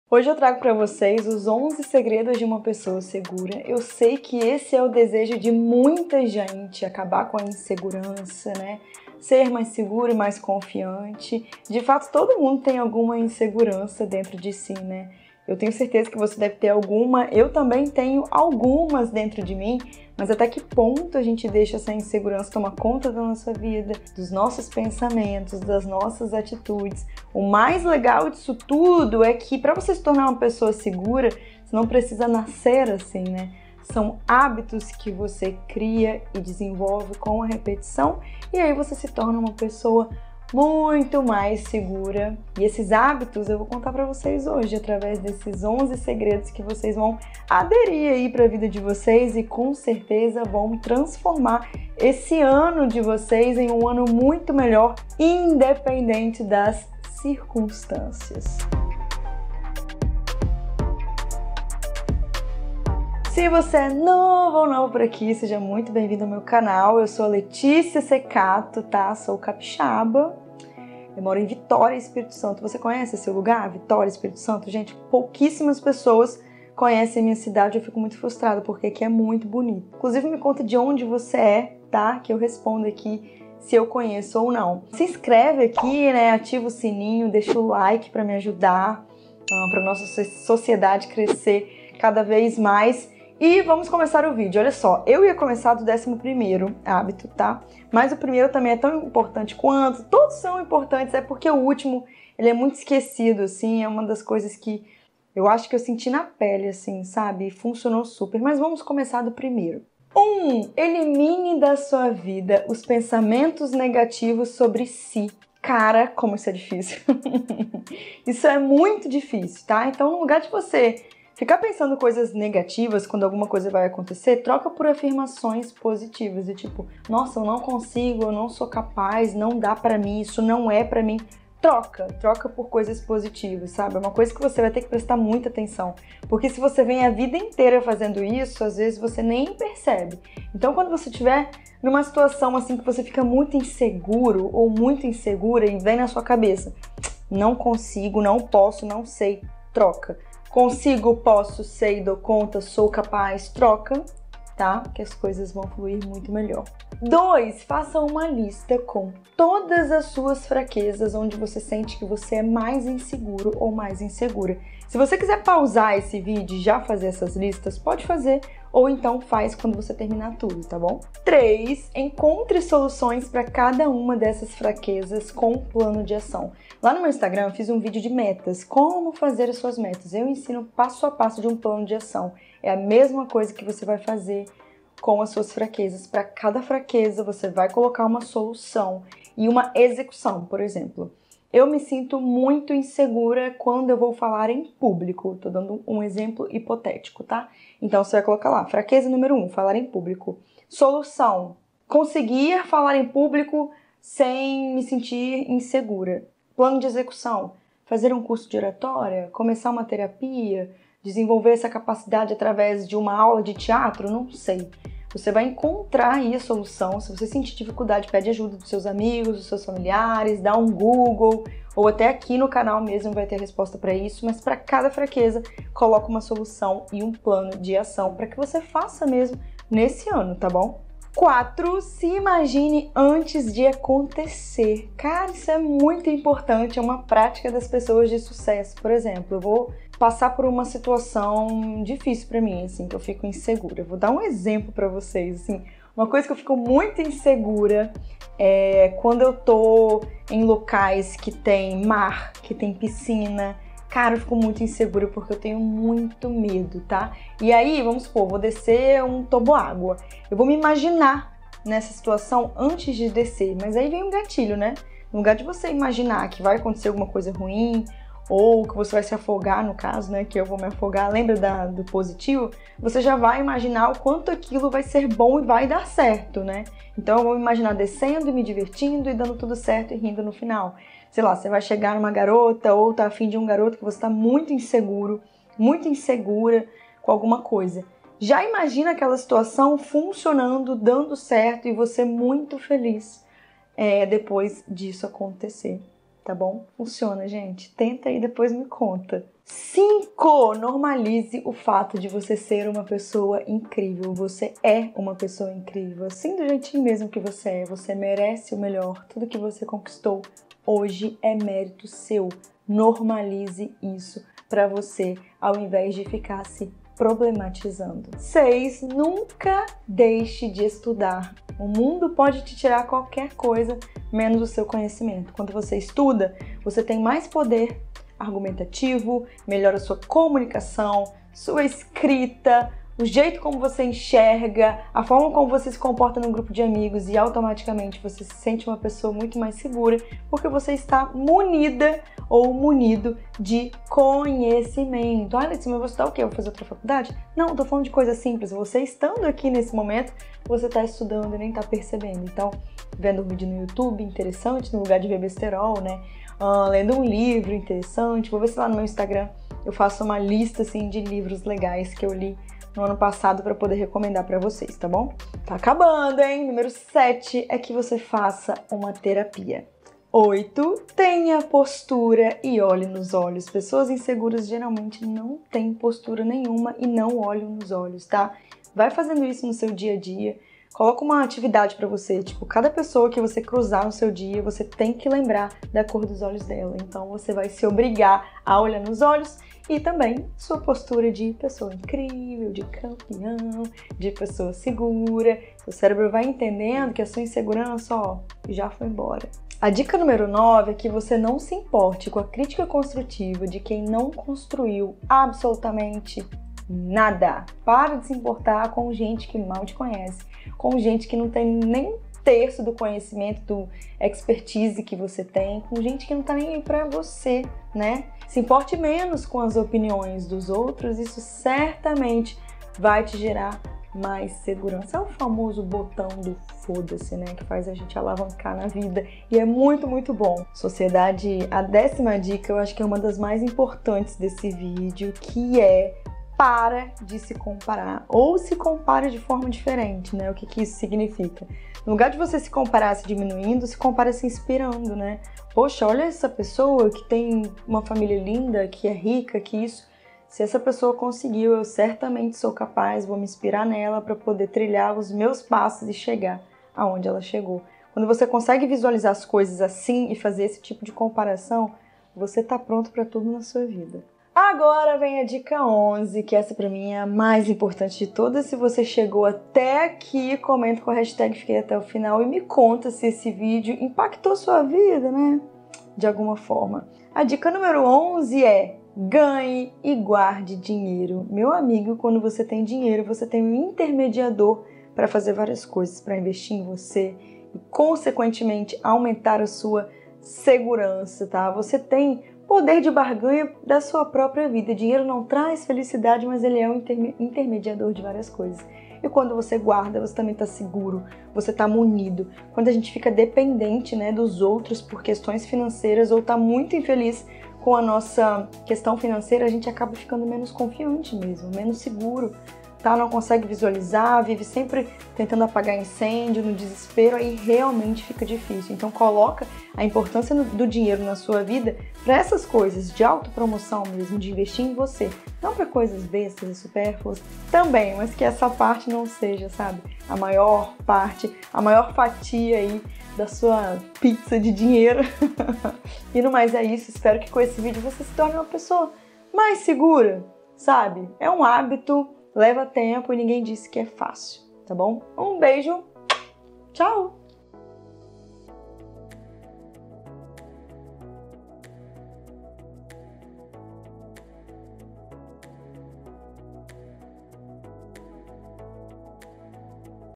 COMO SER UMA MULHER SEGURA?
A insegurança é uma sombra que acompanha muitas pessoas.
Felizmente, é cientificamente comprovado de que perfis comportamentais são totalmente treináveis e qualquer ser humano com força de vontade pode desenvolver.
De fato, ninguém quer ser inseguro e para isso, segredos de ouro precisam ser inseridos na rotina daqueles que querem por um ponto final na insegurança!
Aperta o play e confira o vídeo de hoje;

👉Quer anunciar aqui?